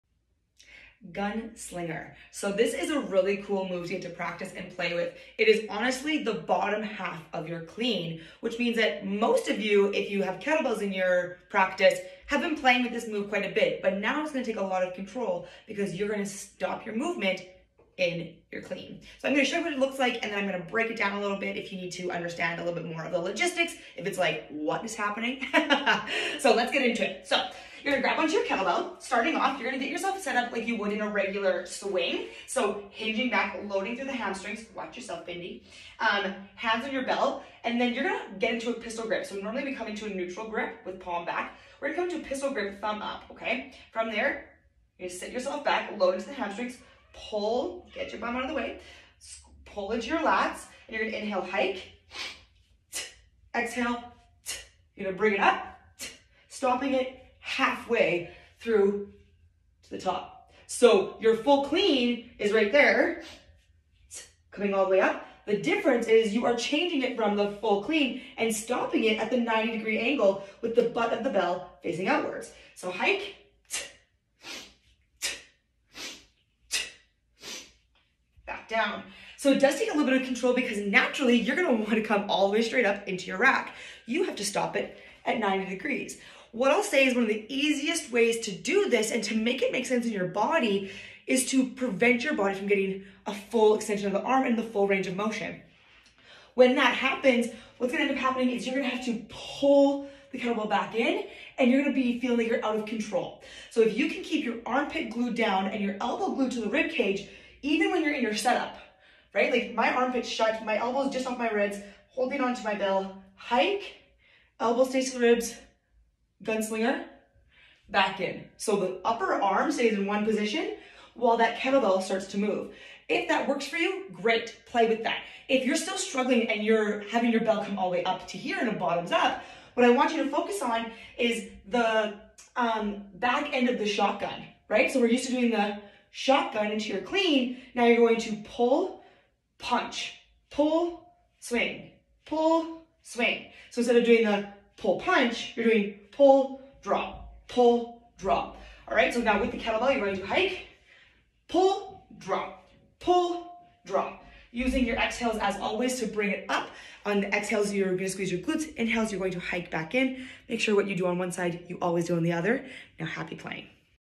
Kettlebell GunSlinger
A fun kettlebell clean variation that takes control and coordination!